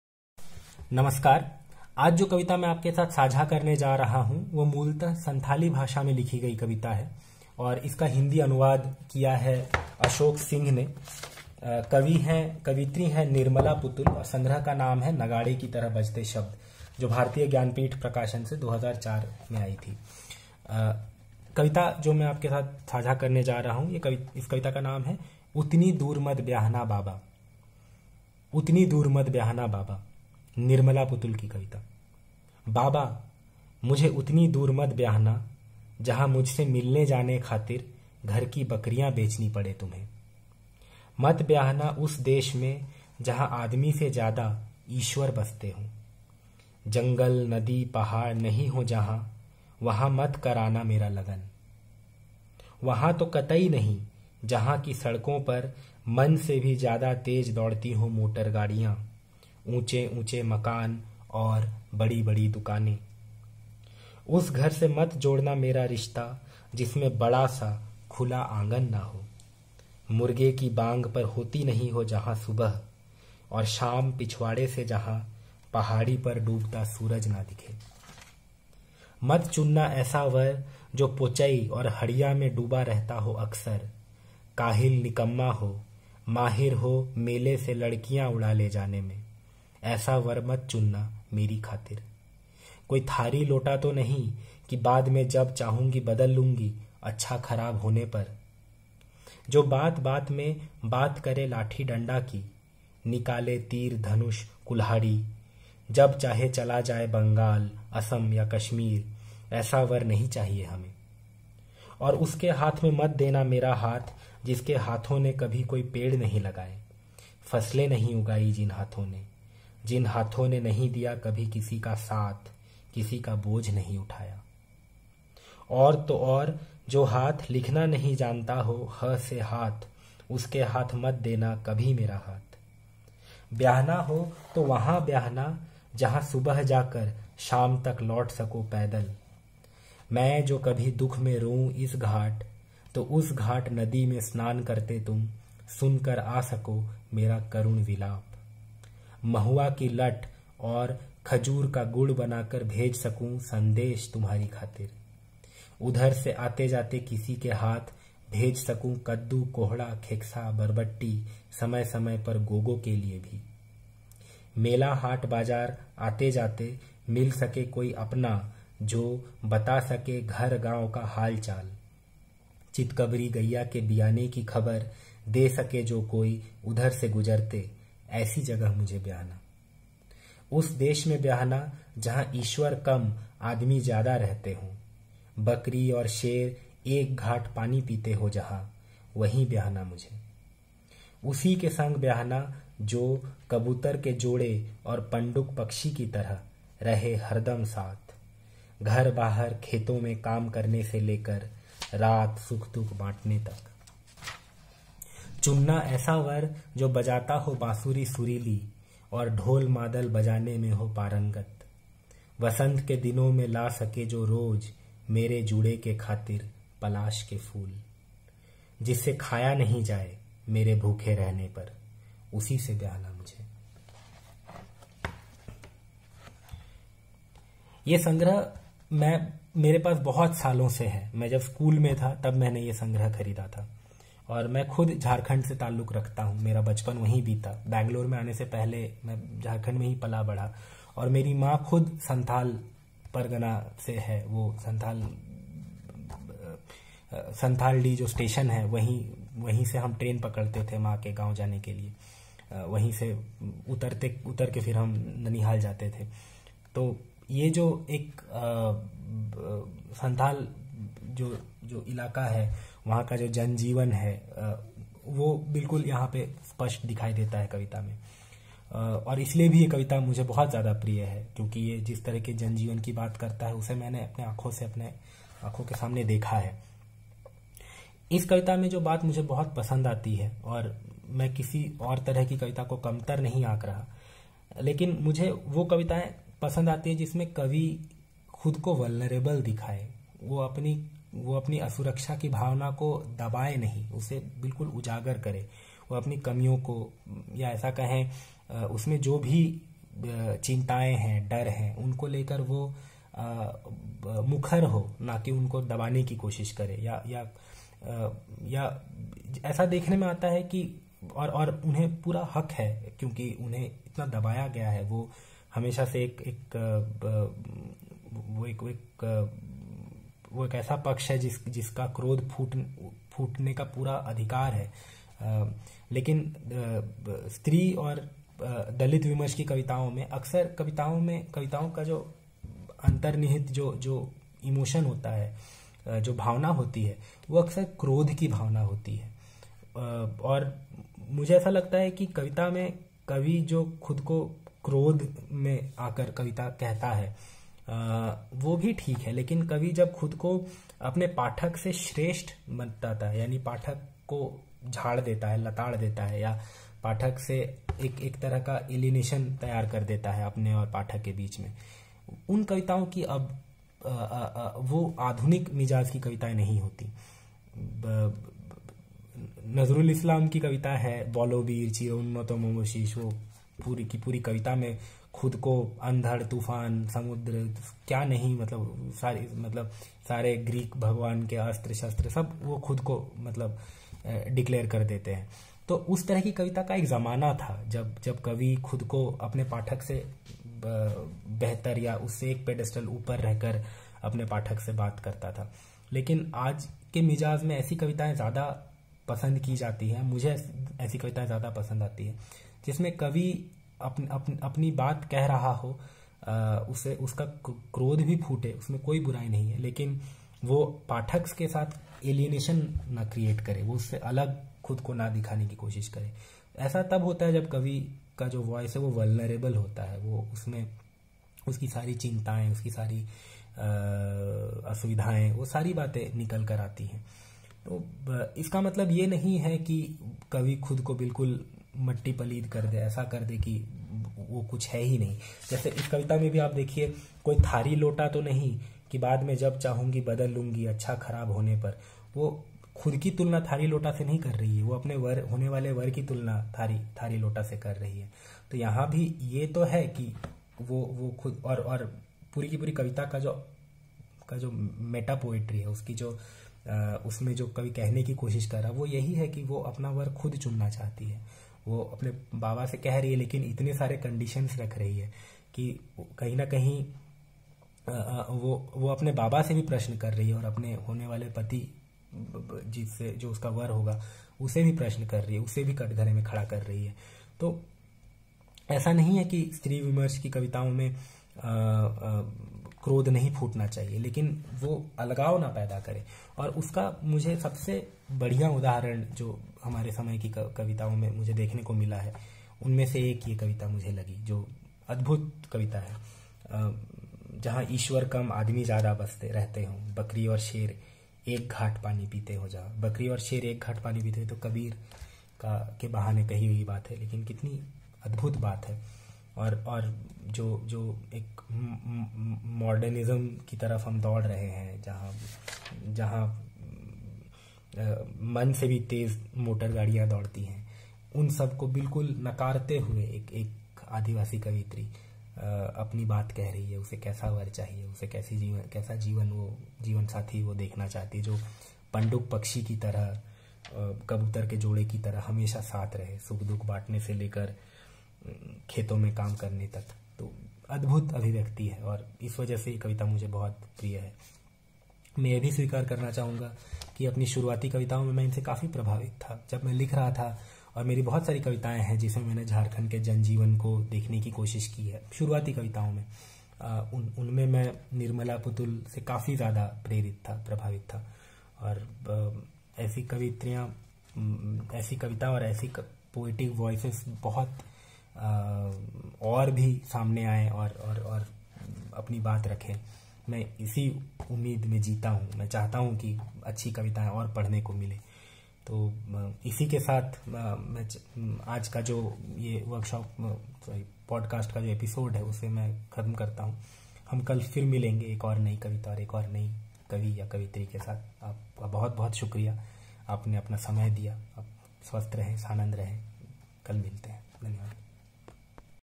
उतनी दूर मत ब्याहना बाबा : निर्मला पुतुल
साथियो, जब तक संभव हो, मैं रोज़ अपनी एक प्रिय कविता आपके सामने प्रस्तुत कर रहा हूँ। इस पॉडकास्ट सीरीज़ का नवीनतम वीडियो।